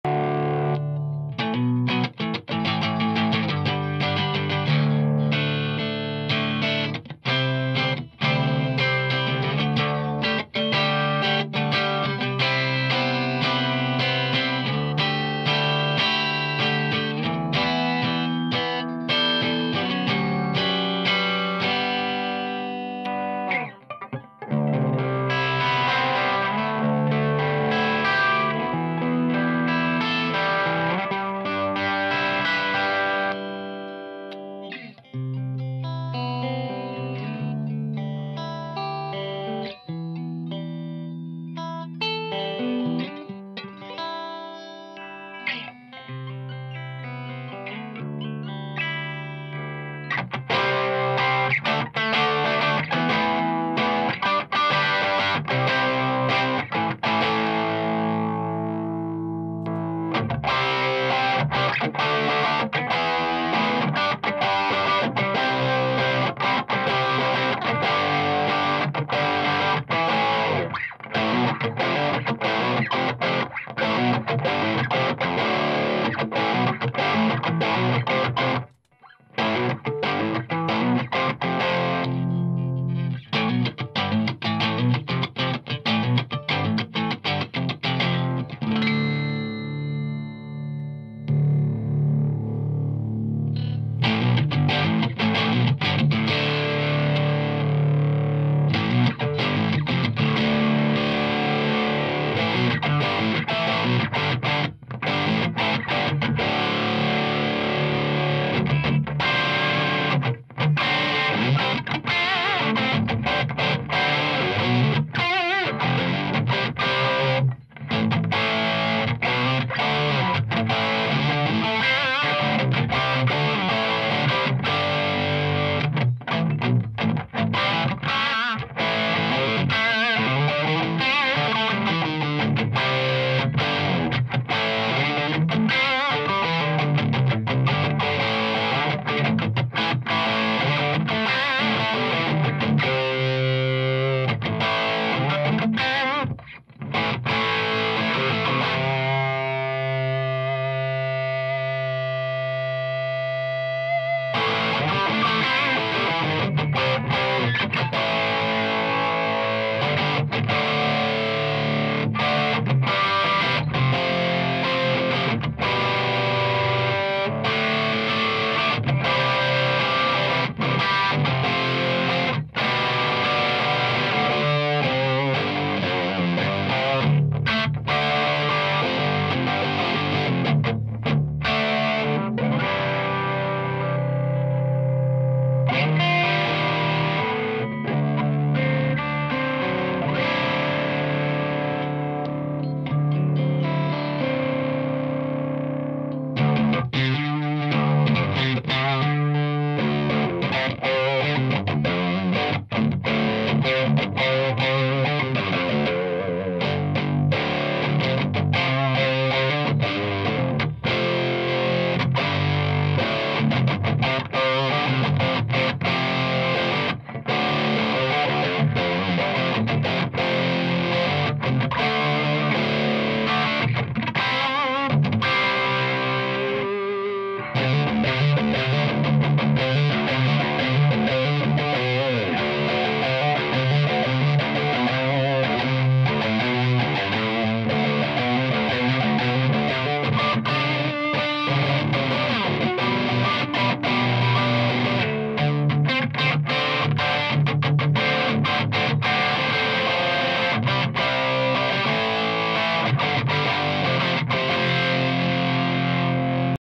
MLC Amplifiers Subzero 9/18
MLC Subzero 9/18
Played for the first time - clean channel + clean with overdrive pedal + crunch channel
with a Gibson Silverburst `78
Played thru 2x12 MLC cab (V30 +G12-65H)